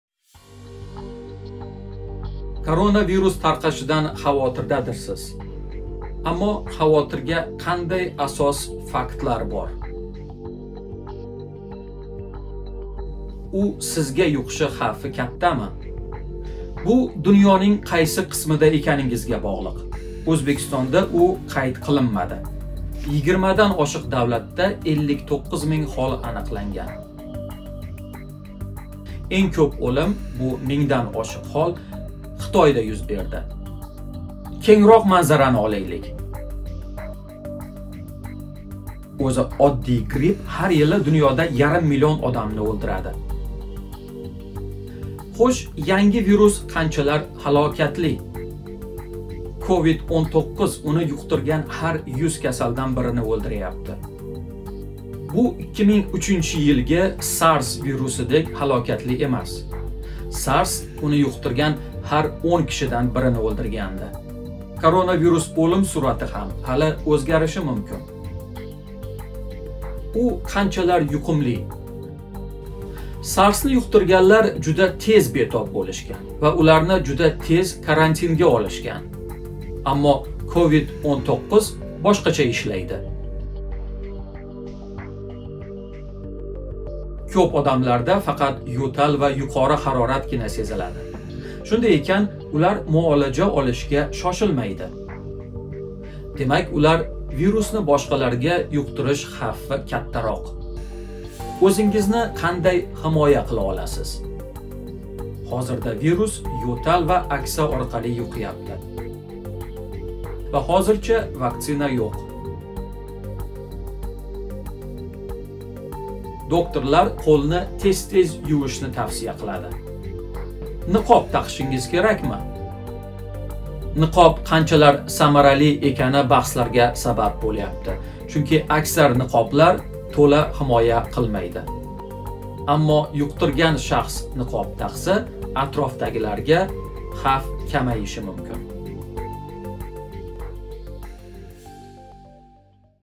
Коронавирус касалидан ўлиш хавфи нақадар катта? - BBC Uzbek O'zbekiston Xitoy
Йигирмадан ошиқ давлатда 59 мингдан ошиқ коронавирус ҳоллари аниқланди. Энг кўп ўлим Хитойда юз берган. Хўш, у нақадар ҳалокатли вирус? Ибрат Сафо